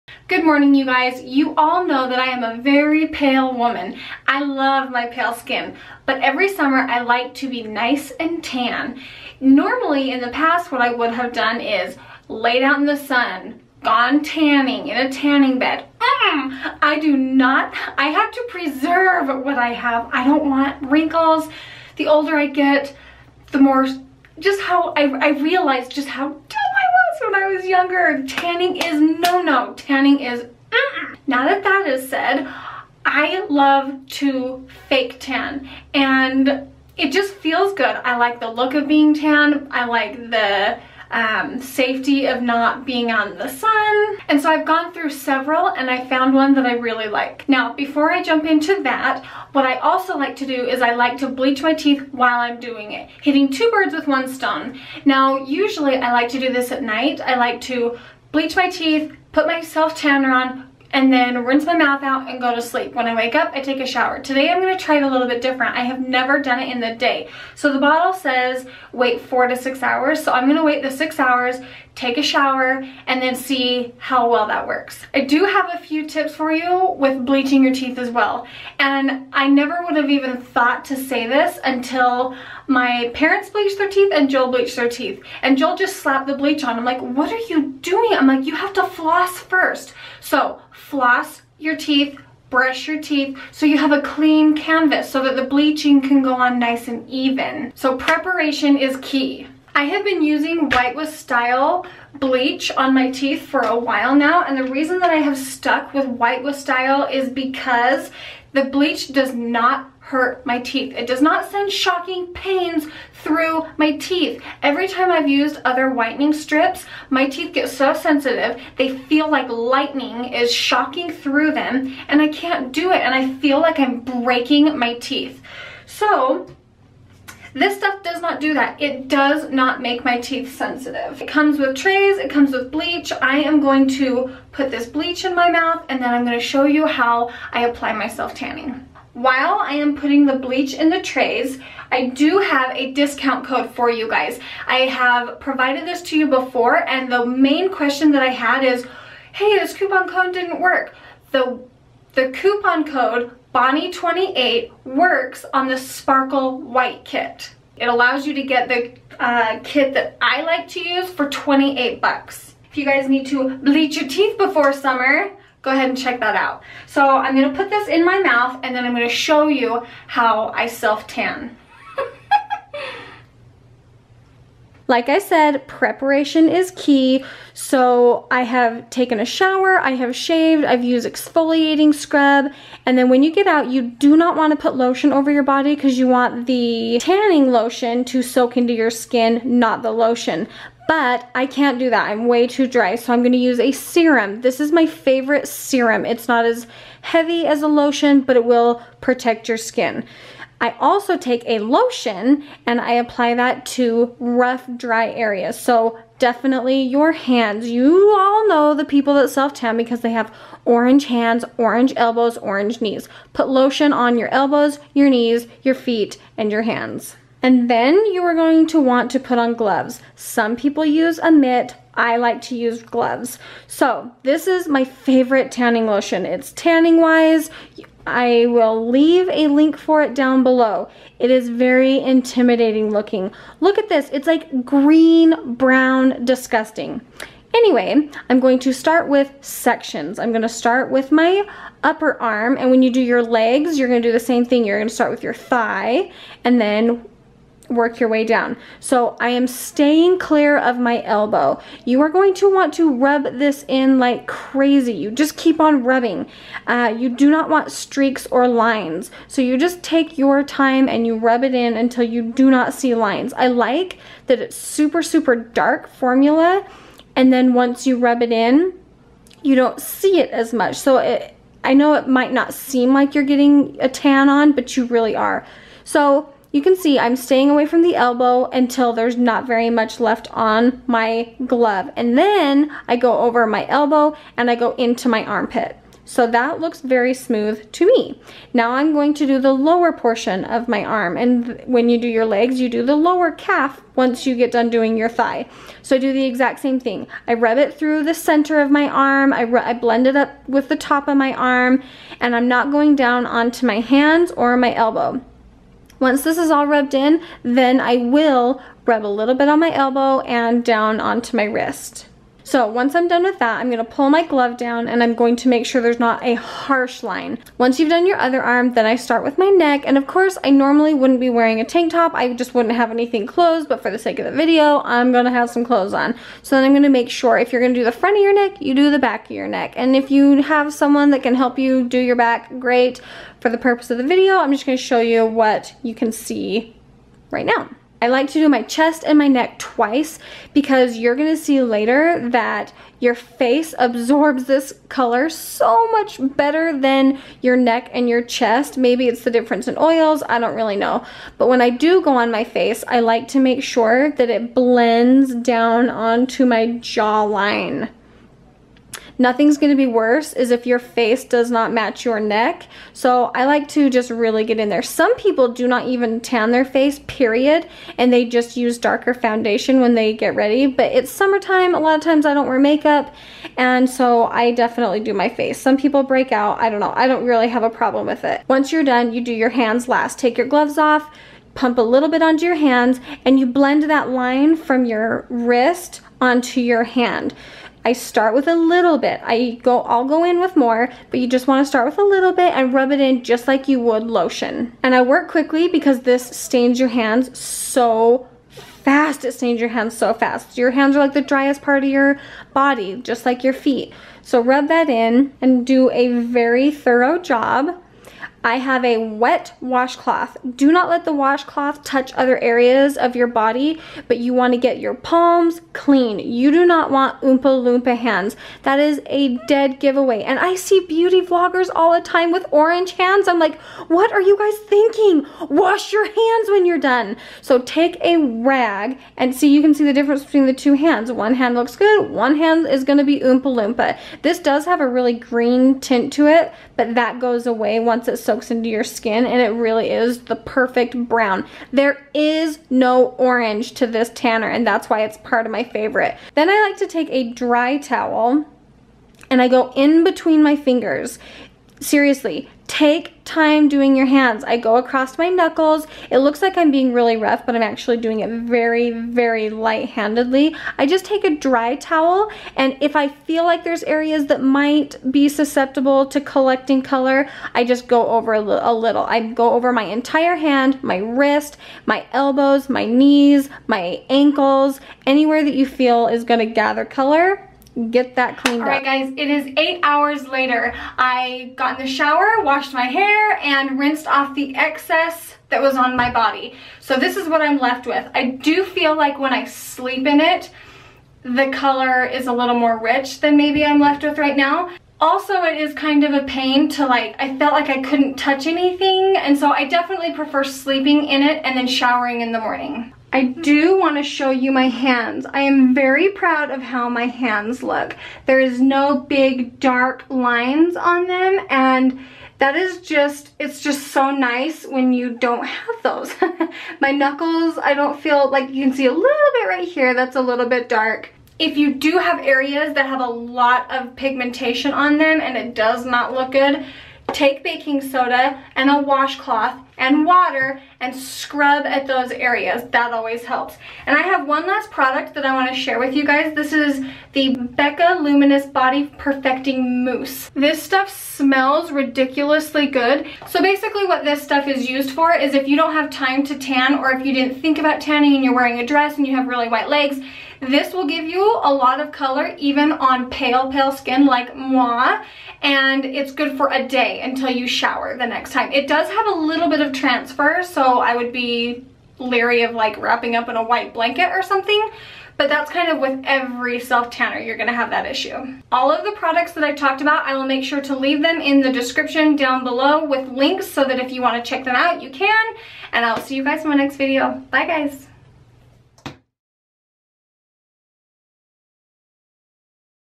Self Tanning and Teeth Bleaching ROUTINE
EVERYTHING YOU NEED TO SELF TAN:

Tanning Mouse
Face Serum
BECCA Skin Perfector

NEW VIDEOS  EVERY :  MON. WED.  FRI.